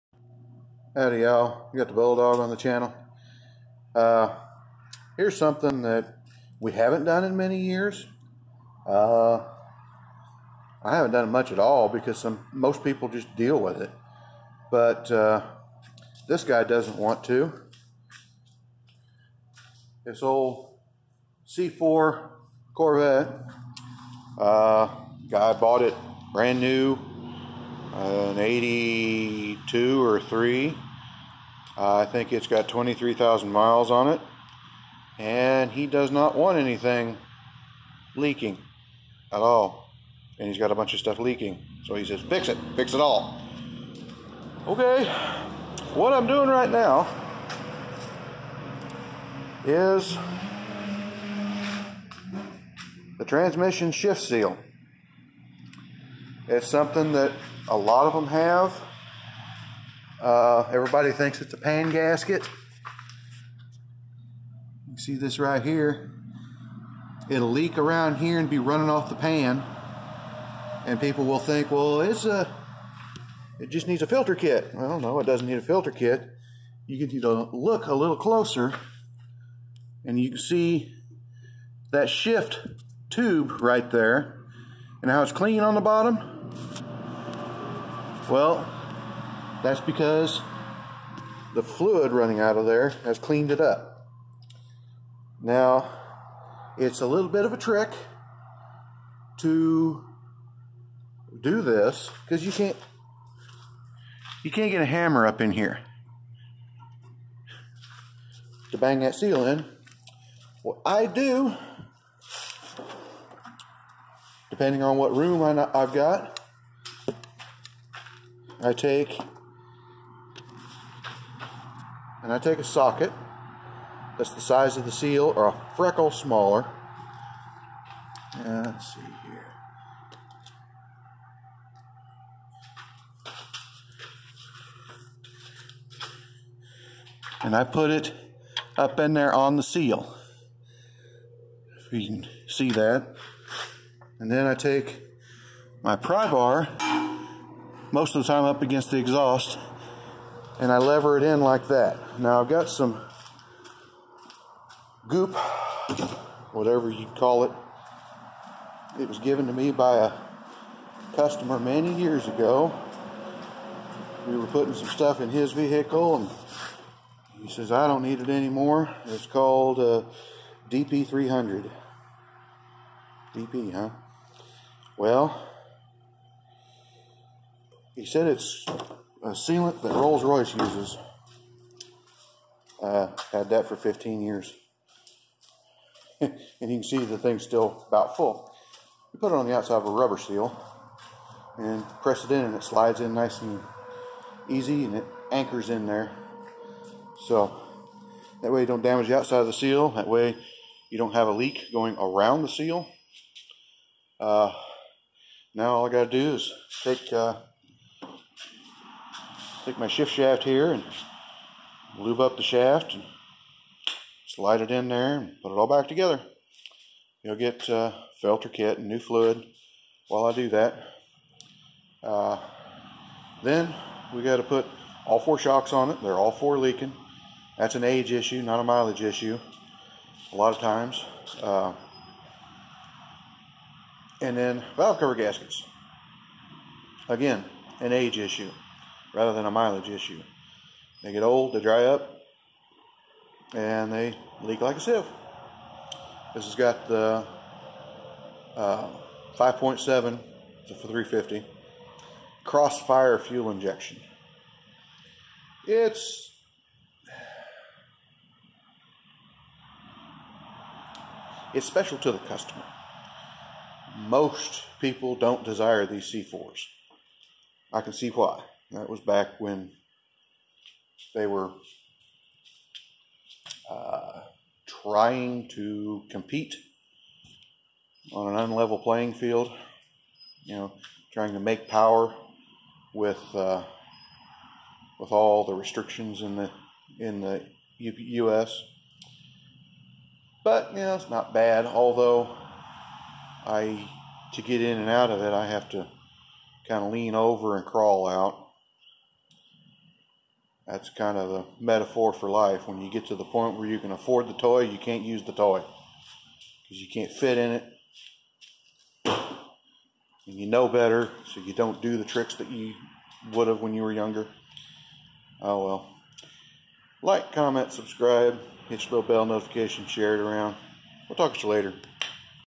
Trans shift seal on a C-4 Corvette
This is a little trick that I use to press in a seal in limited space, this is on a transmission shift seal for the 700R4 in a C-4 Corvette.